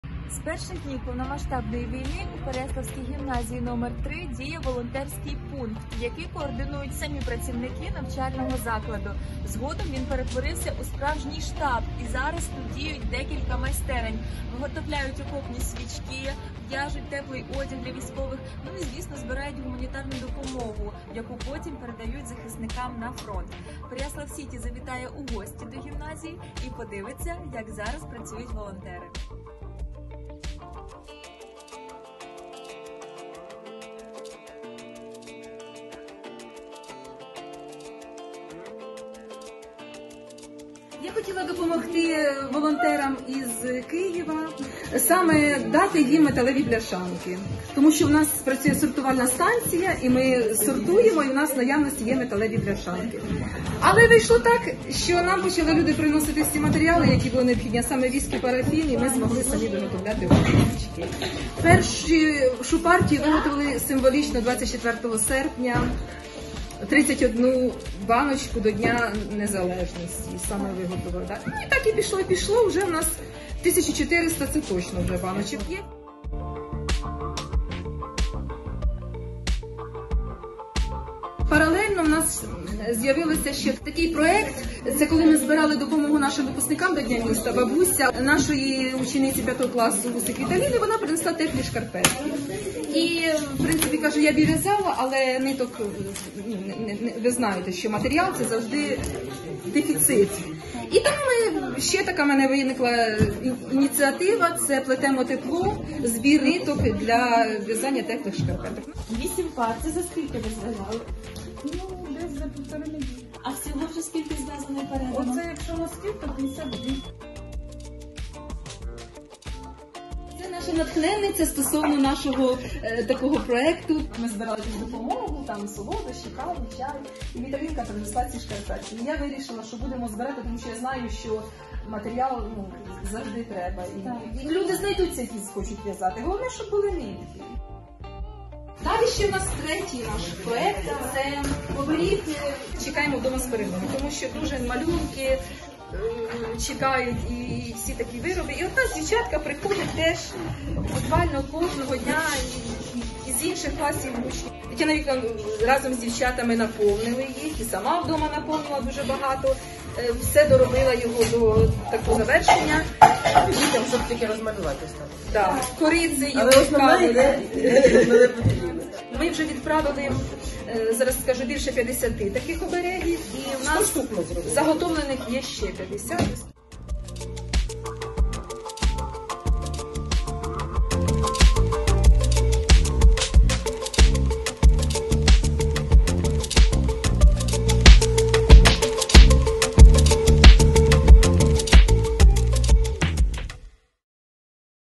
У переяславській школі виготовляють окопні свічки, теплі речі та ароматні обереги для захисників
Понад півтори тисячі окопних свічок, виготовлених педагогами та учнями переяславської гімназії №3, вже відправлені на фронт, щоб зігрівати наших захисників та освітлювати їхні бліндажі. Акція «Гріємо серцем» з кожним днем набуває все більшого масштабу та обростає новими проєктами. Тепер у шкільному «штабі» працюють кілька майстерень.